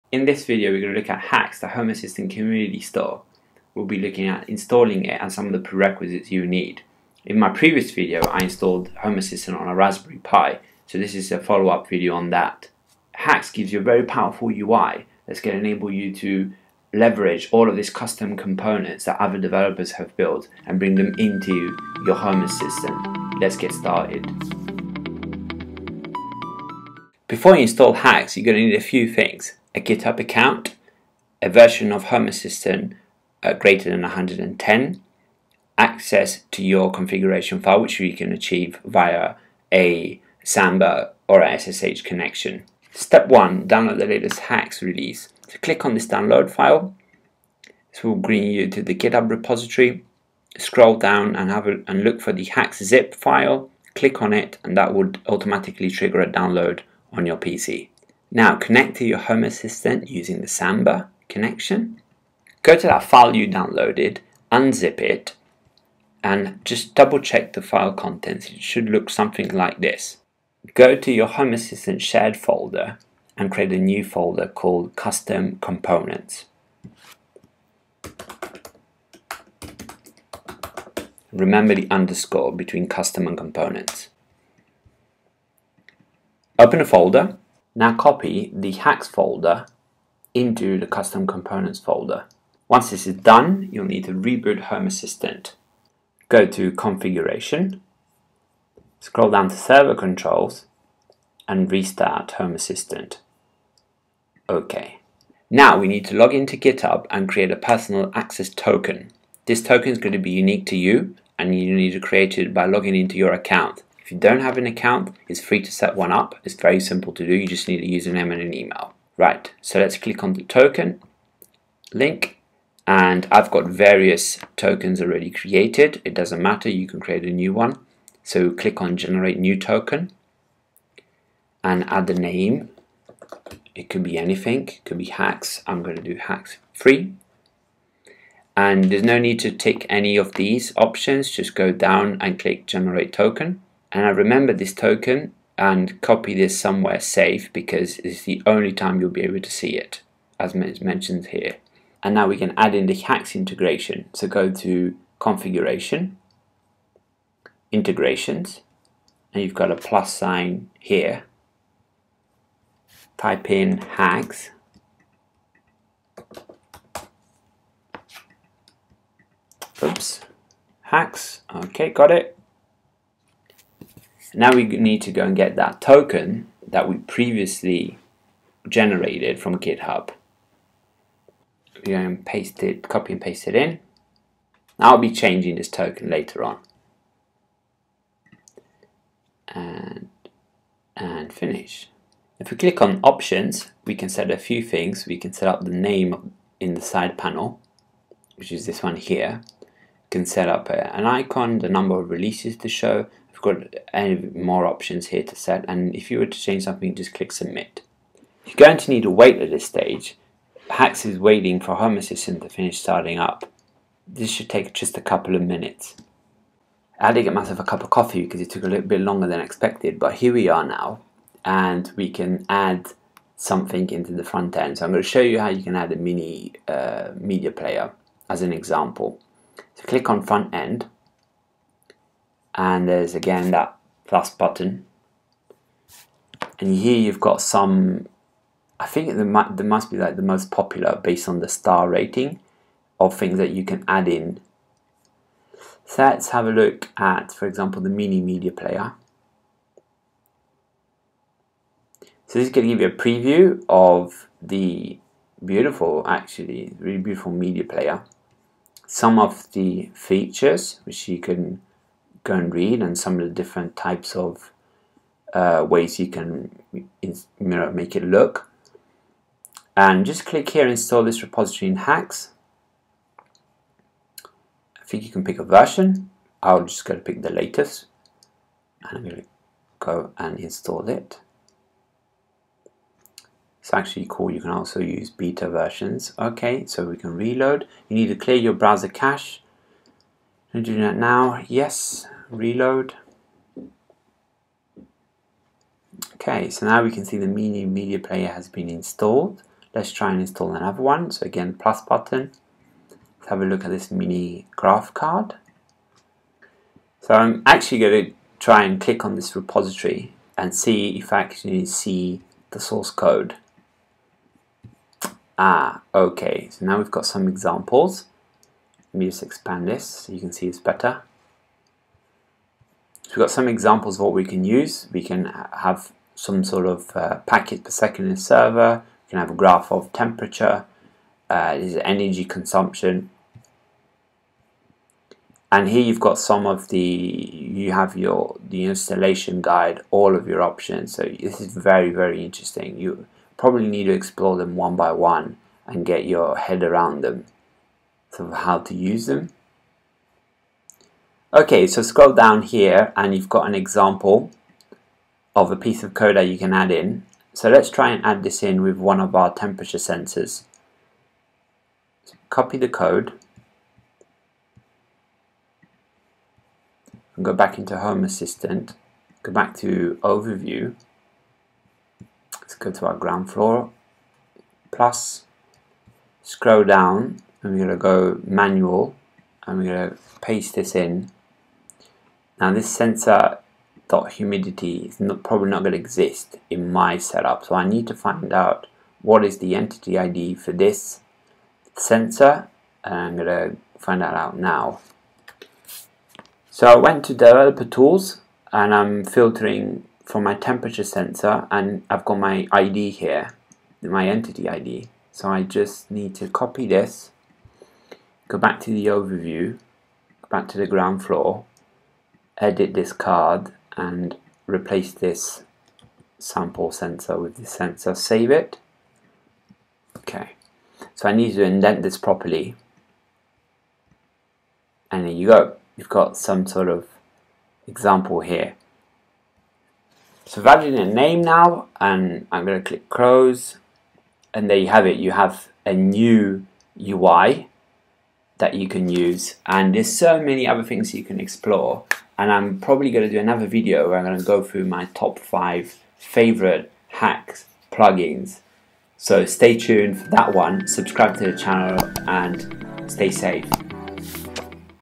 Home Assistant Community Store HACS Installation & Mini Graph Card Configuration
Home Assistant Community Store (HACS) installation with home assistant on a Raspberry PI step by step tutorial. HACS give you a powerful UI to handle download of all your custom needs.

In the video I will also be doing a demo of the mini graph card.